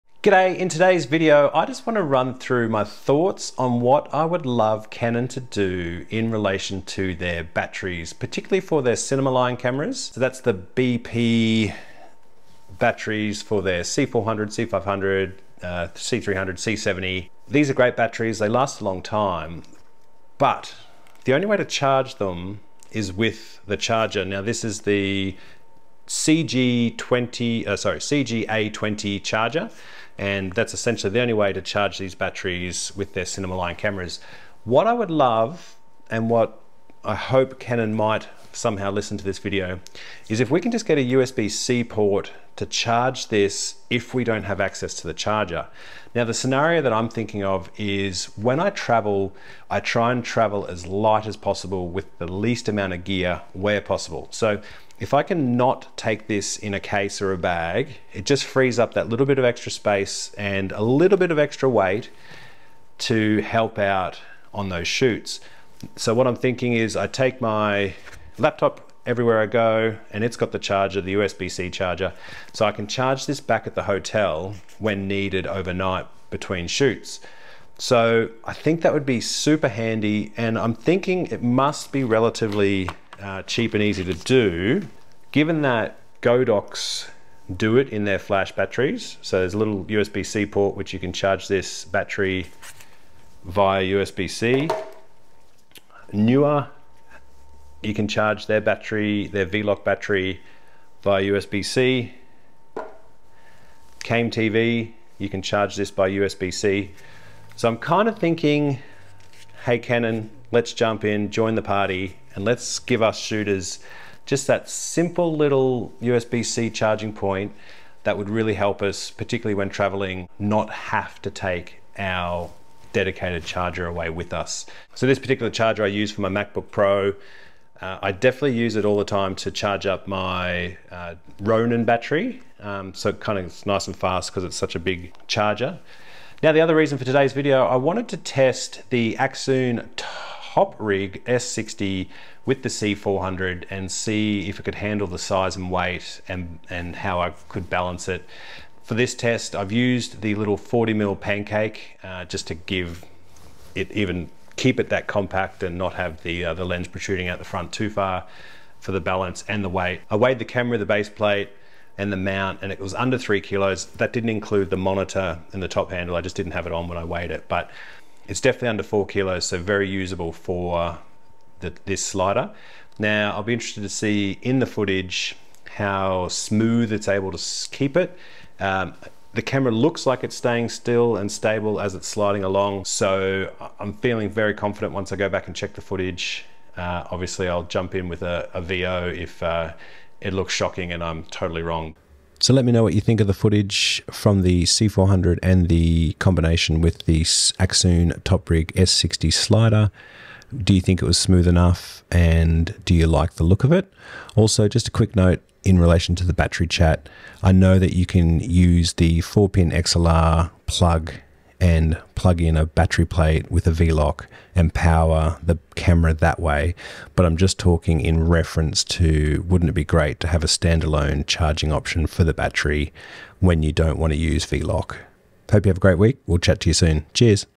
Battery wish + Accsoon s60 slider with C400
See footage from Canon C400 on the Accsoon Toprig S60 slider. Lighting via Godox p600bi and LA Rag House 4x4 Halo frost.

Audio from Sanken Cos-11.  B roll footage from canon r5c.

Tripod Tilta ct12.

00:00 Battery idea
2:14 Accsoon Toprig S60 Slider with Canon C400